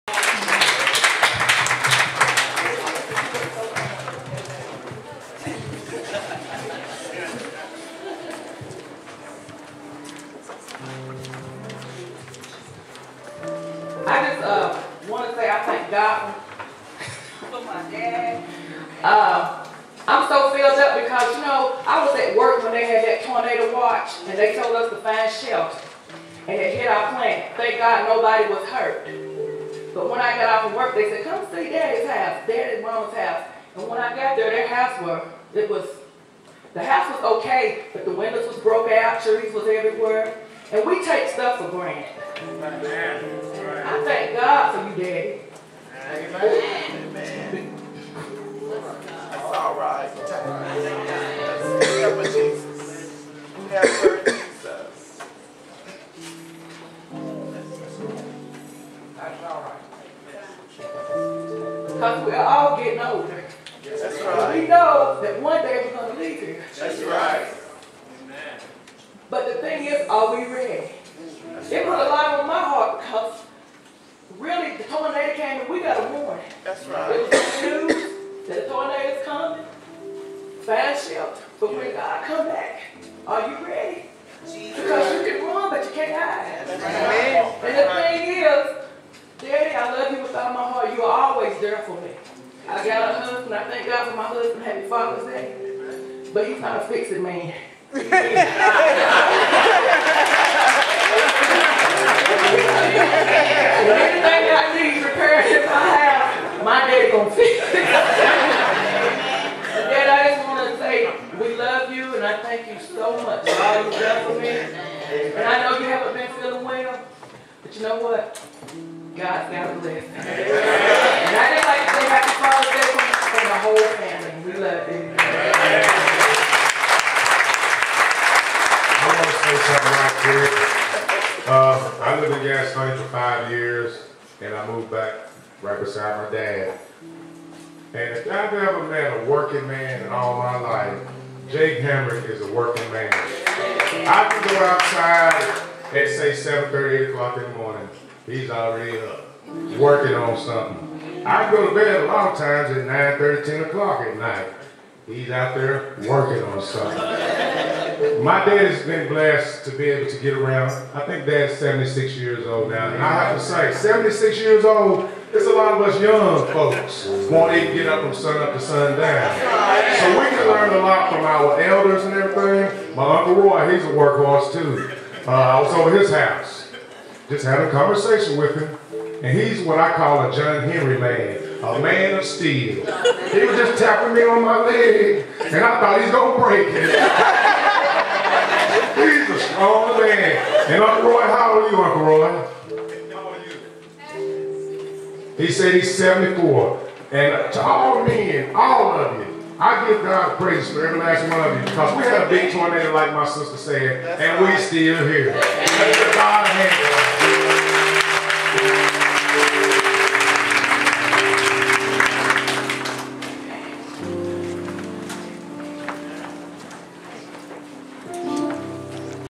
Father's Day Celebration 2009 at Long Branch Church
My sister Gayla Crocker and The Hamrick Family giving Tribute to Jake Hamrick Jr.